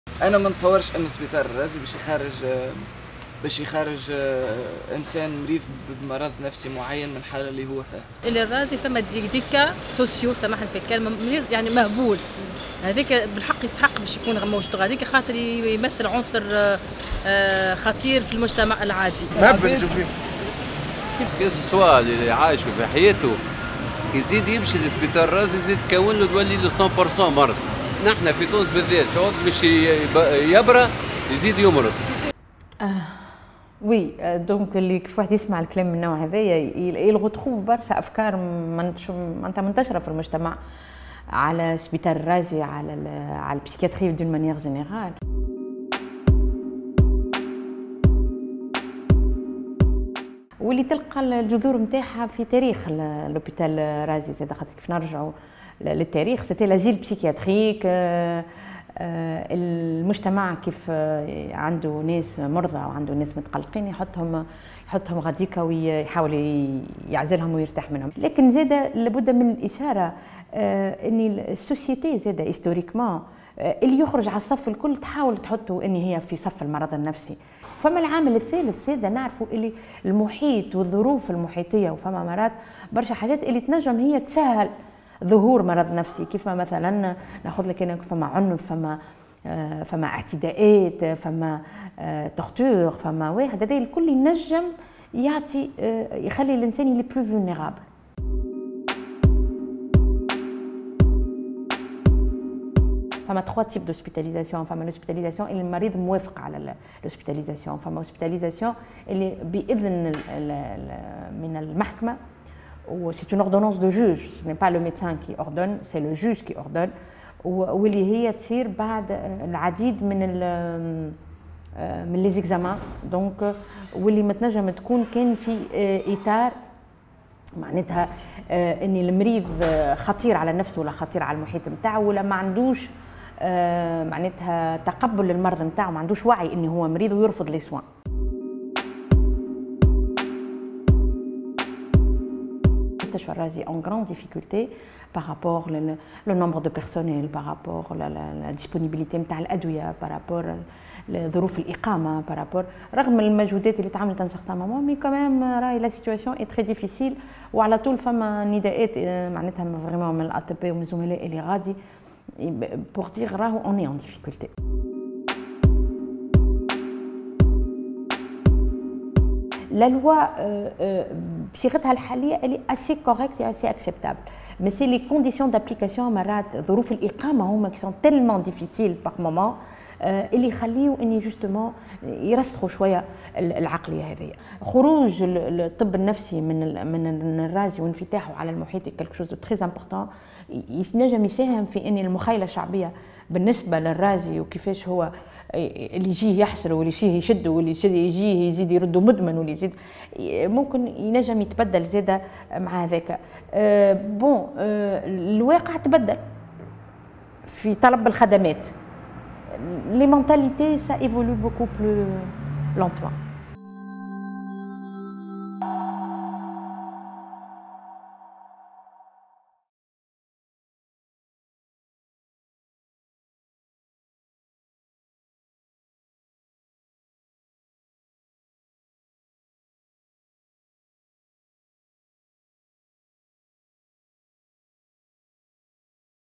مستشفى الرازي والطب النفسي: أحلام بلحاج تفكك الكليشيهات وتفسر الإطار القانوني
يوم 11 جويلية 2018، تمّ إيواء شابة تدعى نهى البشيني بمستشفى الرازي للأمراض العقلية بموجب قرار "إيواء وجوبي" من وكيل الجمهورية بالمحكمة الابتدائية بجندوبة حيث تم الاحتفاظ بها في قسم ابن عمران للامراض النفسية لمدة شهر تقريبا إلى أن أذنت المحكمة الابتدائية بجندوبة، يوم 7 أوت 2018، بتسريحها و بإيقاف الإيواء. و قد شغلت قضية نهى الرأي العام و أثارت جدلا حول ظروف الاحتفاظ بمستشفى الرازي والإجراءت القانونية التي تمكن من ذلك. في هذا السياق كان لنا حوارا مع الدكتورة أحلام بالحاج وهي طبيبة نفسيّة للأطفال بمستشفى منجي سليم.